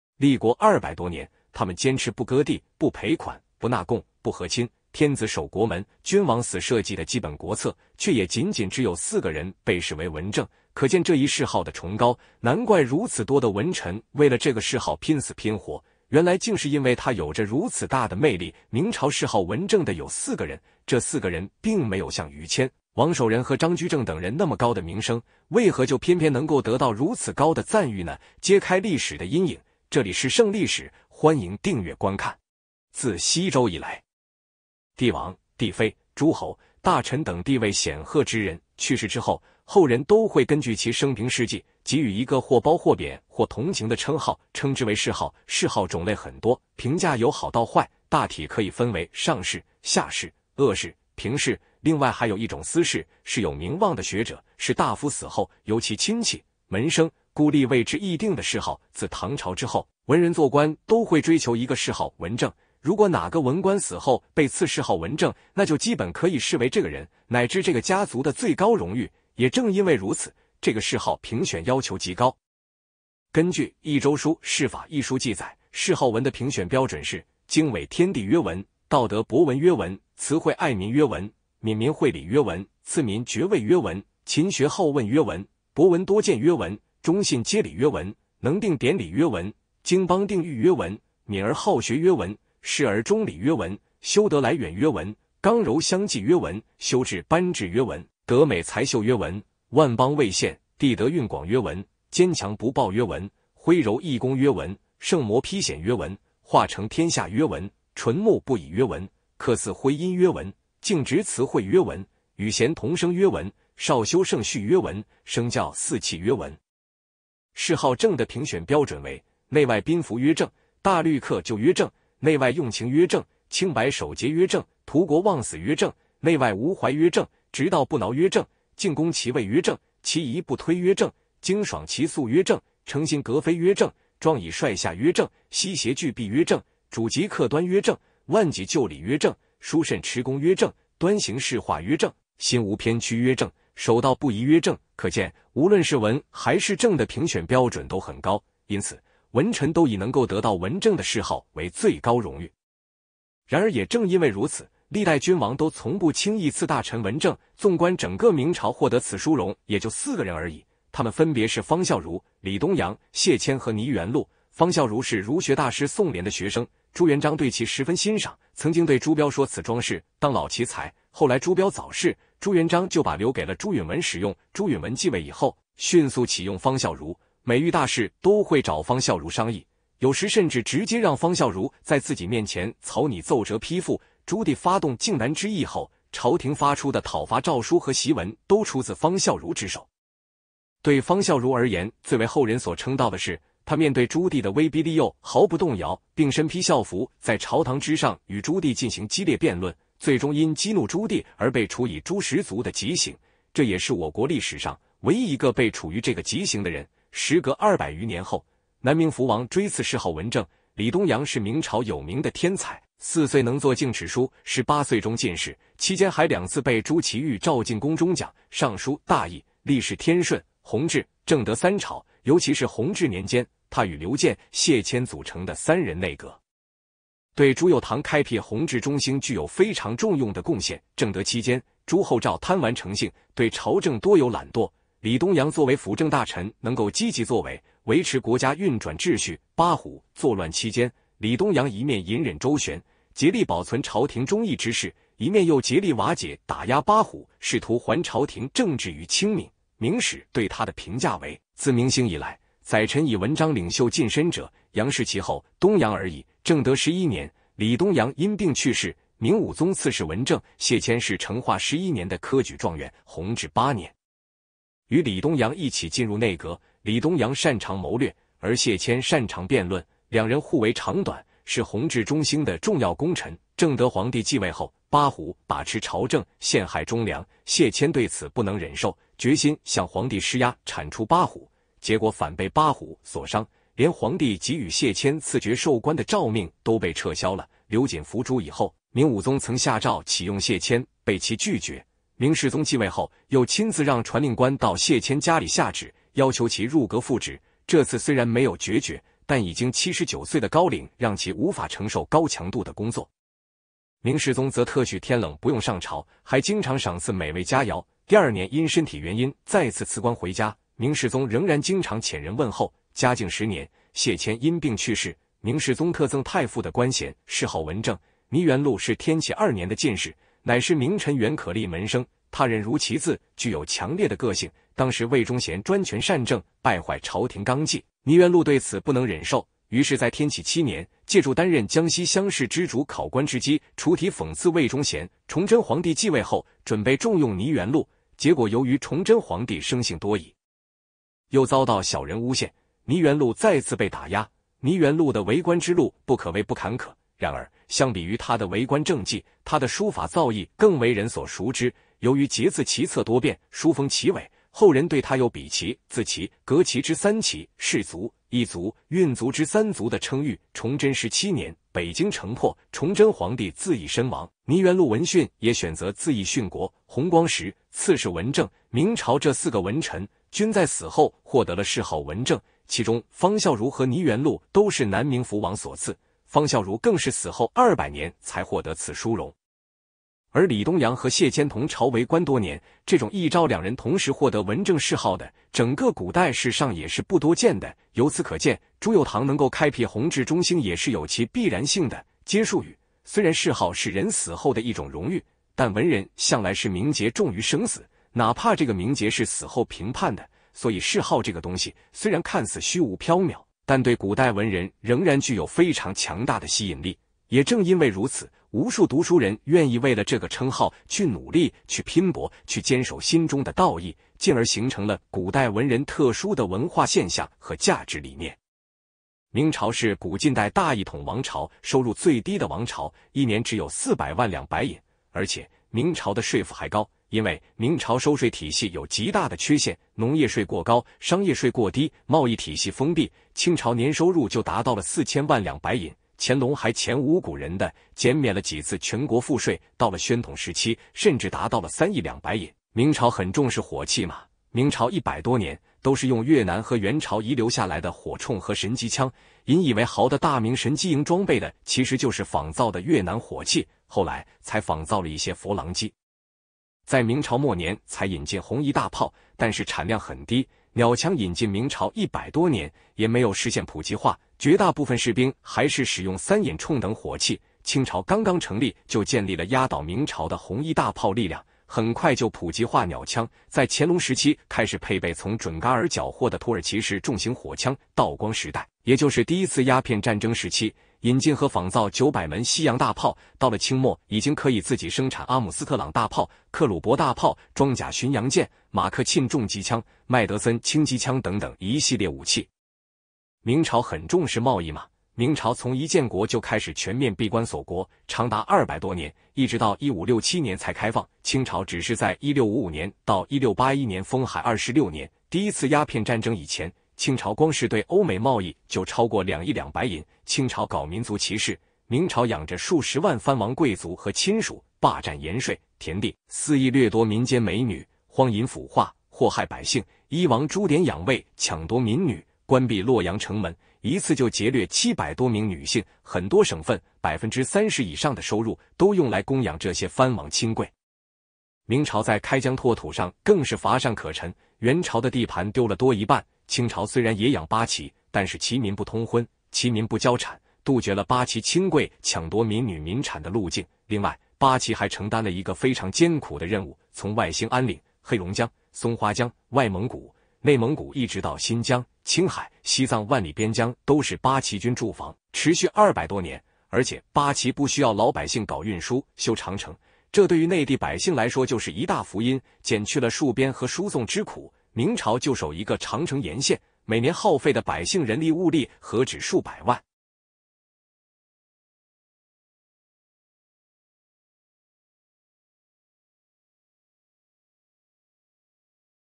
古代文官最高榮譽，整個明朝只有4人獲得，其中2人同朝為官多年 【盛歷史】歷史故事
立國二百多年，他們堅持「不割地，不賠款，不納貢，不和親，天子守國門，君王死社稷」的基本國策，卻也僅僅只有四個人被謚為「文正」，可見這一謚號的崇高，難怪如此多的文臣為了這個謚號拼死拼活，原來竟是因為它有著如此大的魅力。明朝謚號「文正」的有四個人，這四個人並沒有像於謙、王守仁和張居正等人那麽高的名聲，為何就偏偏能夠得到如此高的贊譽呢？

本影片旨在解說社會新聞和奇聞異事，絕對符合YouTube社群準則。